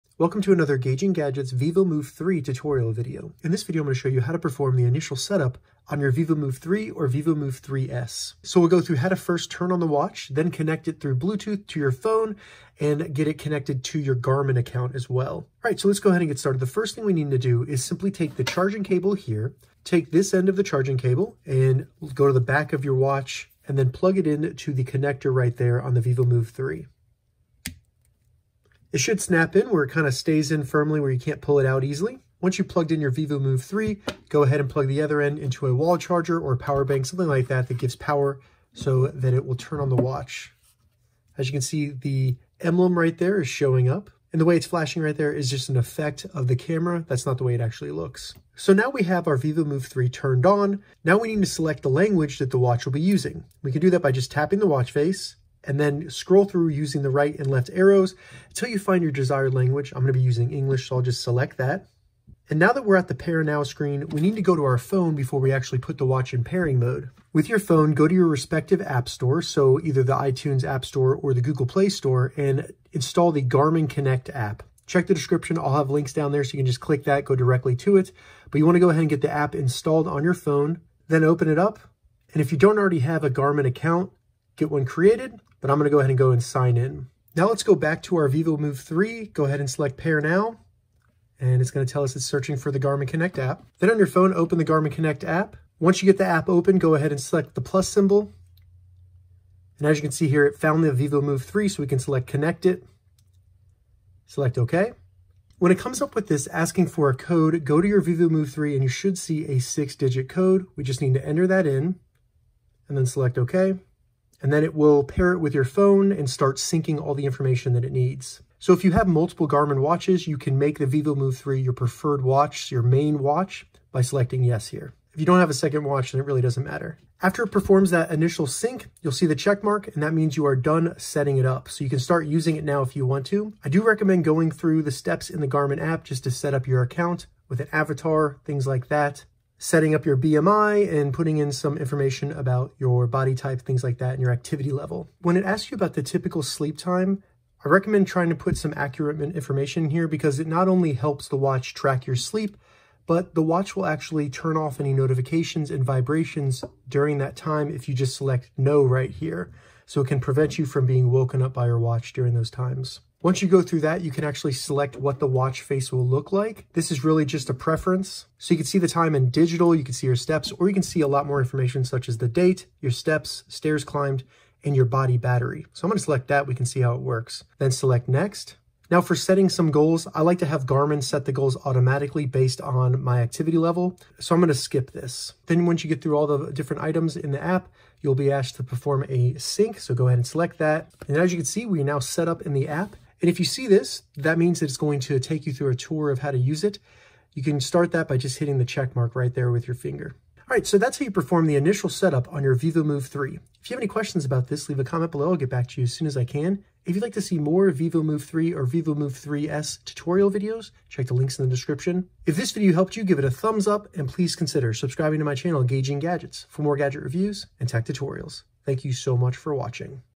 How to Turn On and Setup the Vivomove 3 - Garmin Vivomove 3S, Luxe, Style
Just get a Garmin Vivomove 3 watch? In this tutorial, I show you how to set up your Garmin Vivomove 3, 3S, Luxe, or Style for the first time. This tutorial will go though turning on the Garmin Vivomove 3, Installing the Garmin Connect app, pairing with your phone, and personalizing the Garmin Vivomove 3 for you.
Vivomove 3 on Amazon(paid link)

Follow the steps below to setup your Garmin Vivomove 3:
1. Turn on the Garmin Vivomove 3 by connecting the charging cable and then charging the Vivomove 3.
2. Navigate to your respective app store to download and install the Garmin Connect App
3. Open the Garmin Connect app and create an account.
4. Ensure your bluetooth is enabled then select the plus symbol in the app to set up a new device
5. If your Garmin Vivomove 3 is on the app should automatically find your watch. If not find the Garmin Vivomove 3 in the list of available watches. Select Connect it.
6. Check the Garmin Vivomove 3 for a pairing code. Enter that into the app when prompted
7. Go through the setup steps for sleep time, wrist, Goal setting, and device syncing.
8. Your Garmin Vivomove 3 has now been set up and is ready to be used. Enjoy!